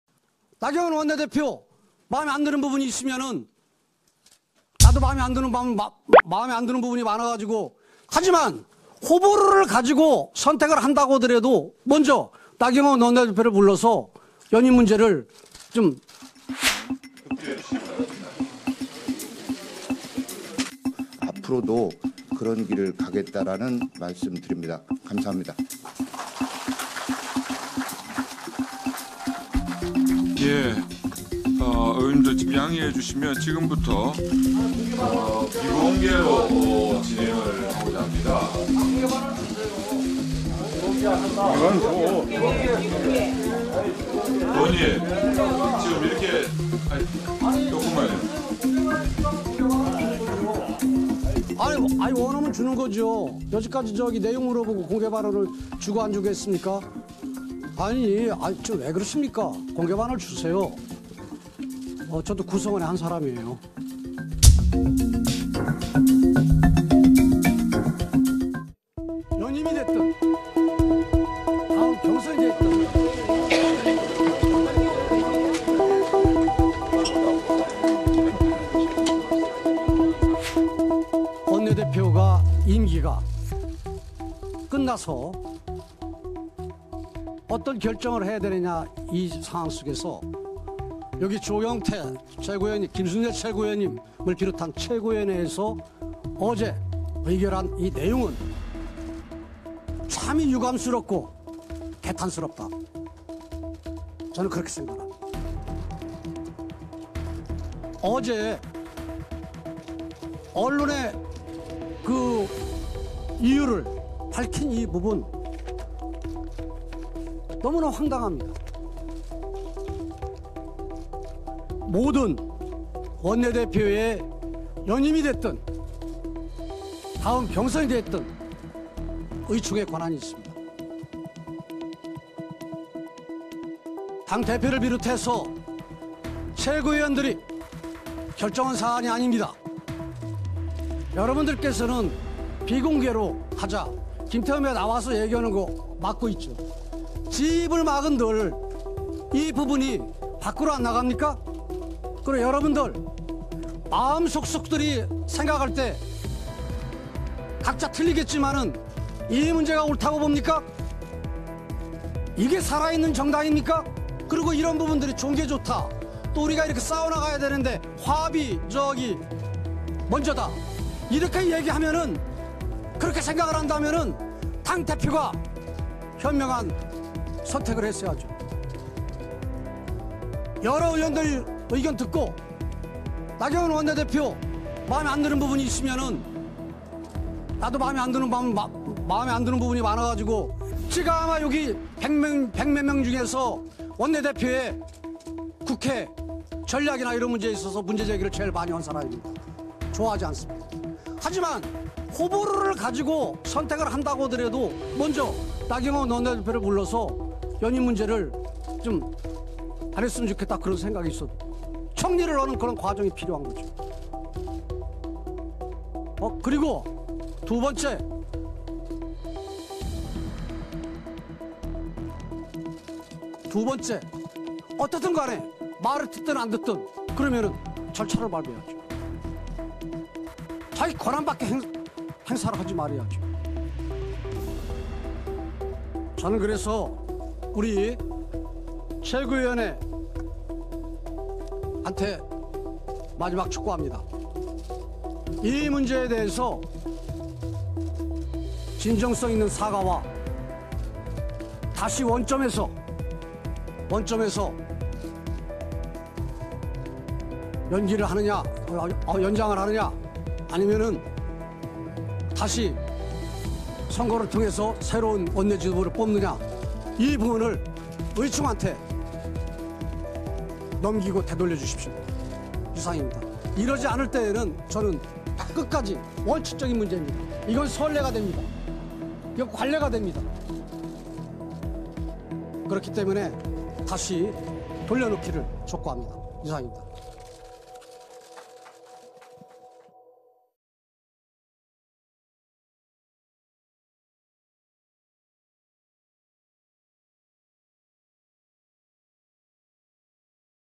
황교안 저격한 김태흠 "나도 나경원 안 좋아하지만…최고위 결정 개탄" (현장영상) / SBS
김태흠 자유한국당 의원이 나경원 원내대표의 임기 종료를 결정한 황교안 대표와 최고위원회를 강하게 저격하고 나섰습니다.
김태흠 의원은 오늘(4일) 국회에서 열린 한국당 의원총회에서 "나도 나 원내대표를 안 좋아하지만 황 대표가 나 원내대표에게 뜻을 묻고 의원들의 총의를 모으는 과정이 필요했다"고 목소리를 높였습니다.
사회를 맡은 이만희 의원이 의원총회를 비공개로 전환하려고 하자, 김태흠 의원은 손을 들고 공개발언을 요구하며 단상 앞으로 걸어나갔습니다.